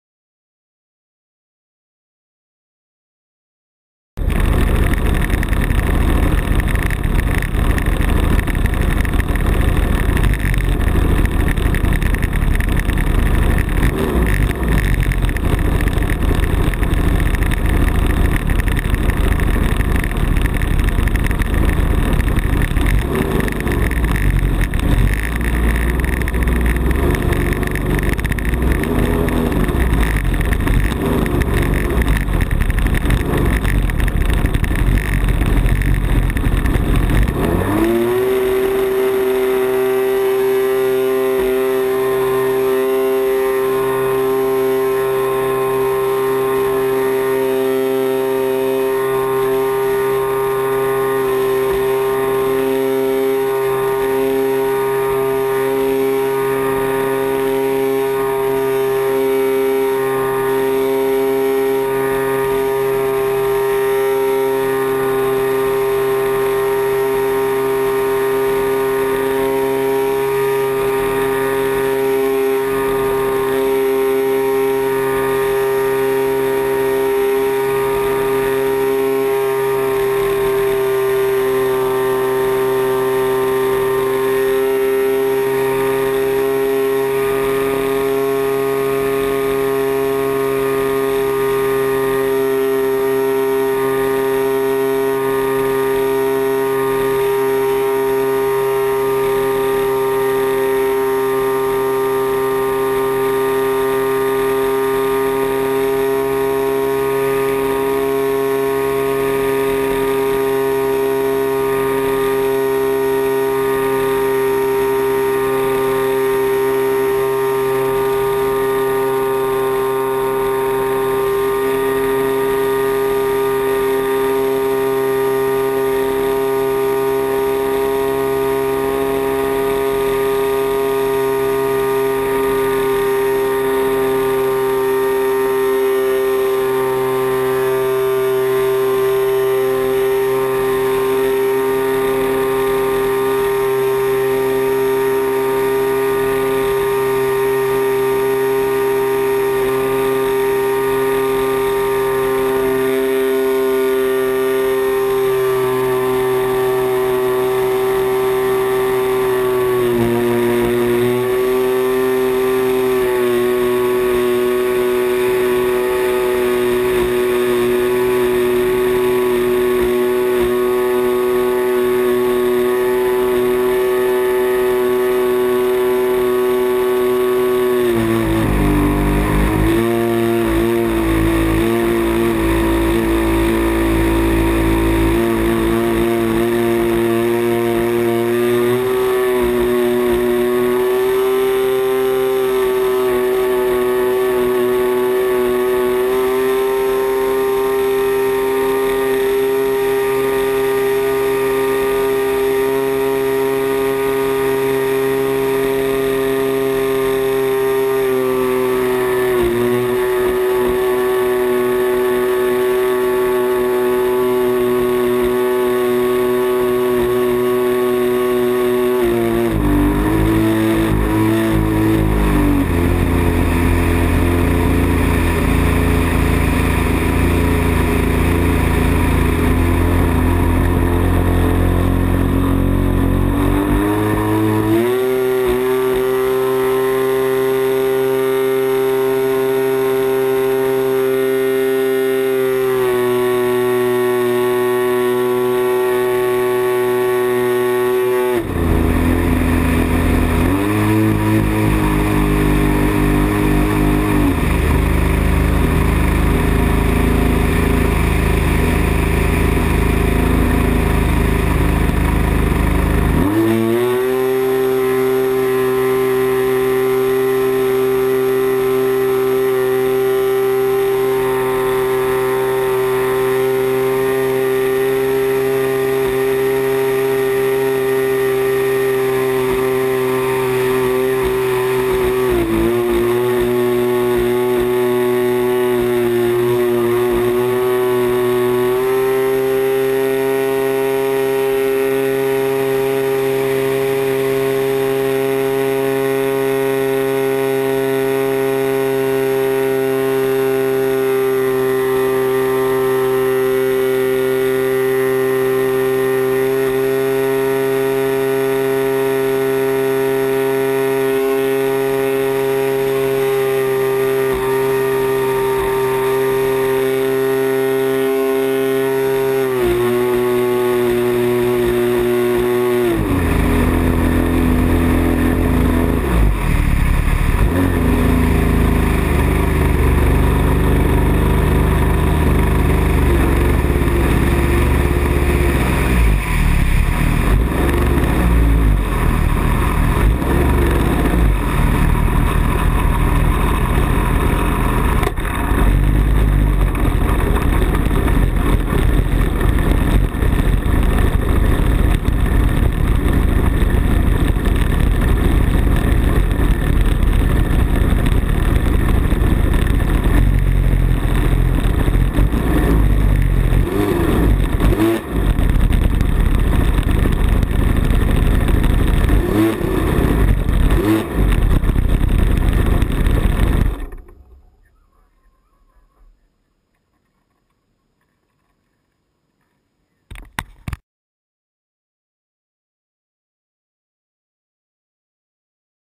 Great Planes Giant BIG Stik with GoPro Hero 2 Camera on board.wmv
Flying my Giant big stik with my new GoPro Hero 2 Camera on board of this giant bird. Worked well, did not relize I had my flaps up during the first few minuties of the flight. I love this camera, and this plane! 40cc CRRC engine.